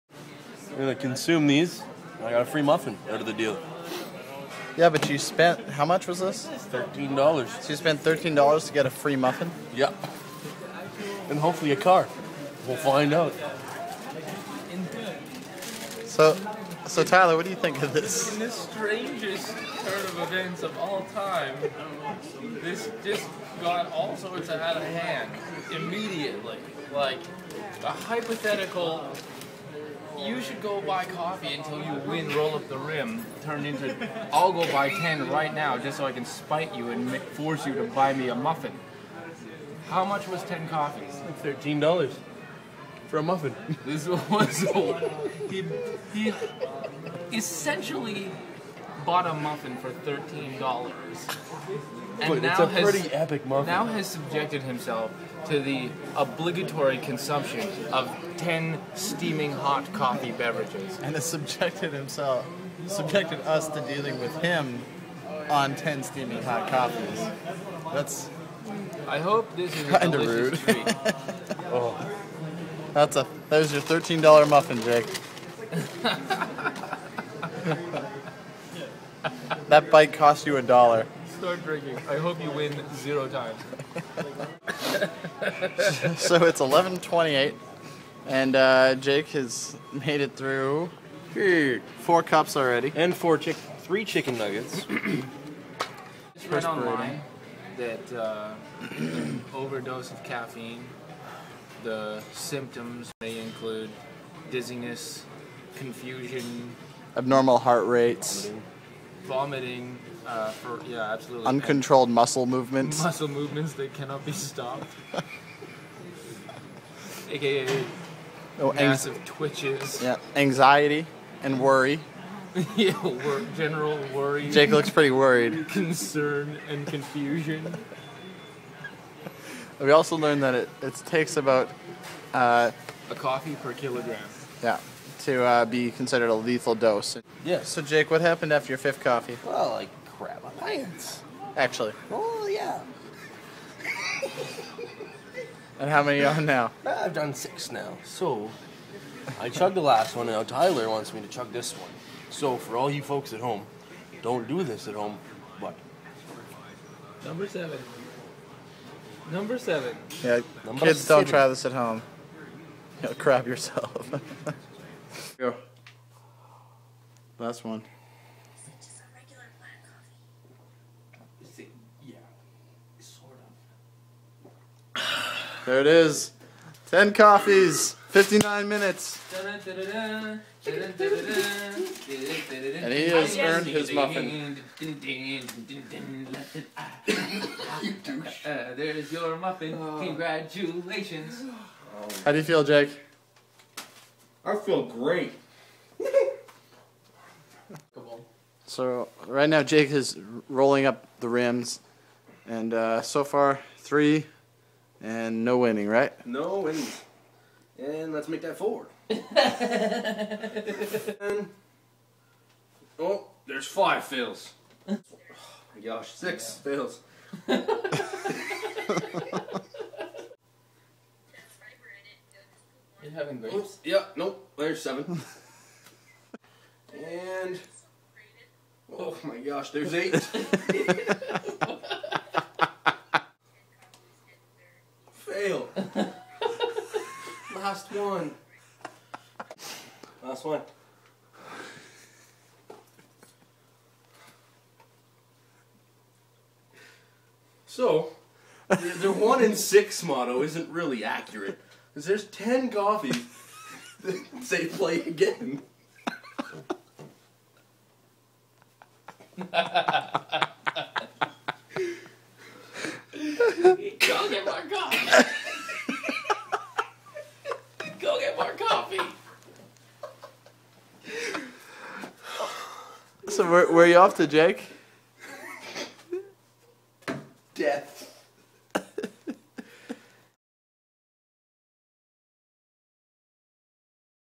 Kiros Roadie Roll up the Rim
Kiros roadie Jake drinks 10 coffees at Tim Hortons in an attempt to win something.